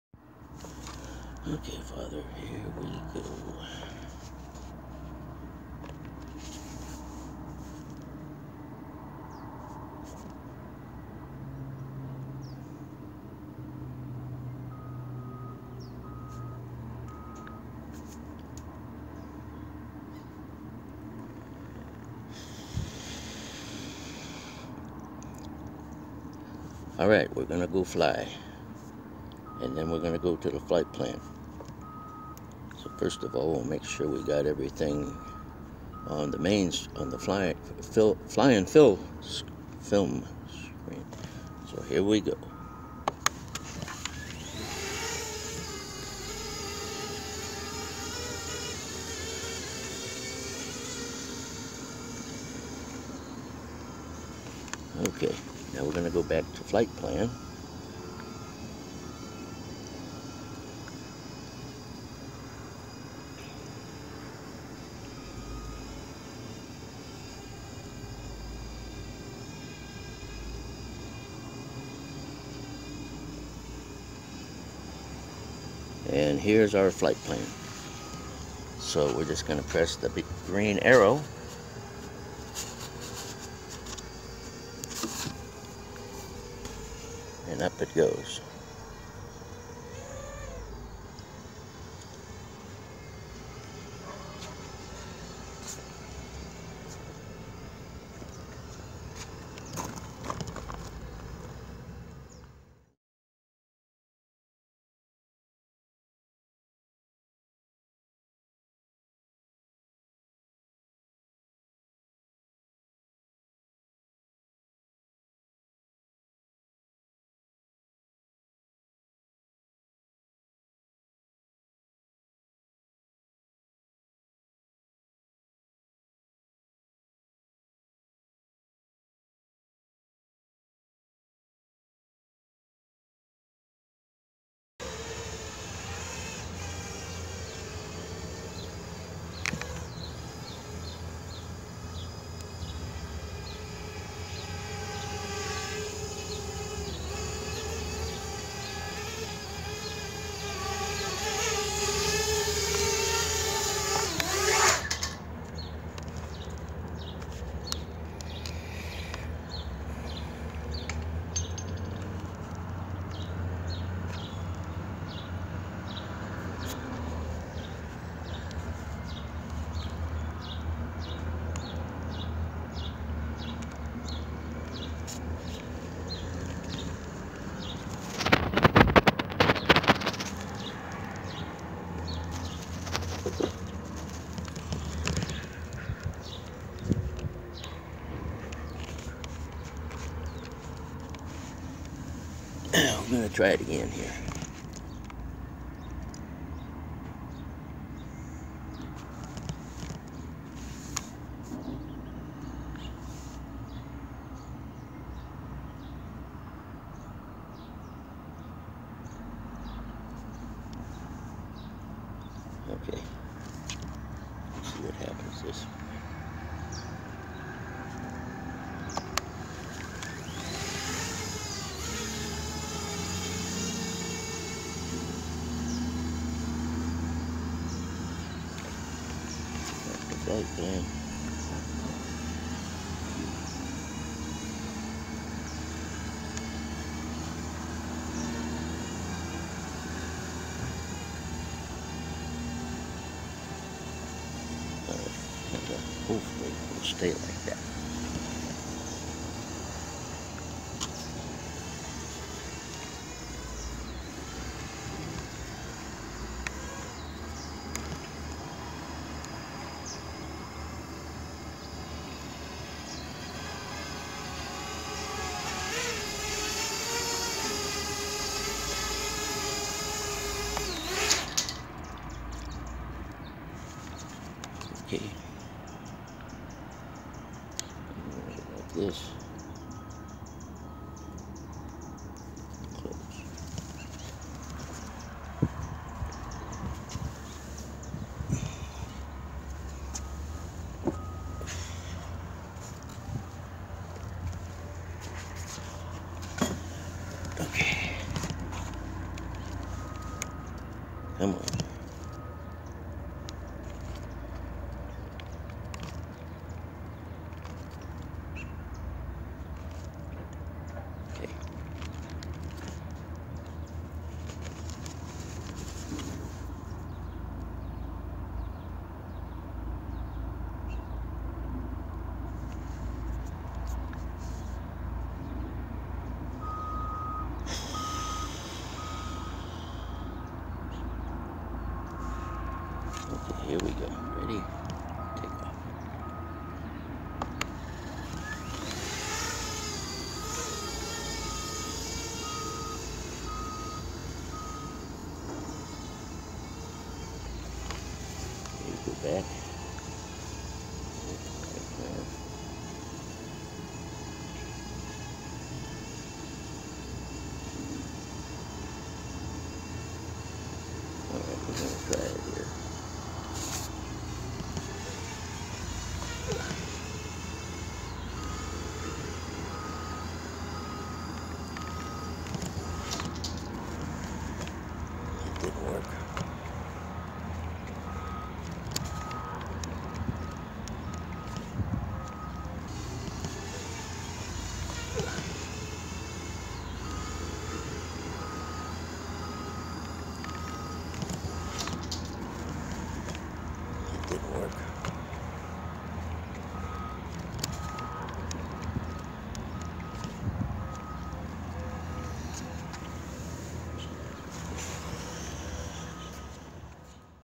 THAT STUBBORN BEBOP 2 FLIGHT PLAN
When we press the “start” arrow in the FreeFlight Pro app, That plan remains in memory until it is “finished” or everything is rebooted. I discovered this by mistake trying to execute the plan I created for the complete tutorial recently uploaded. Fortunately I had kept a record on my tablet of all the issues I had trying to start that flight plan over.

This video is that record. Enjoy and God bless.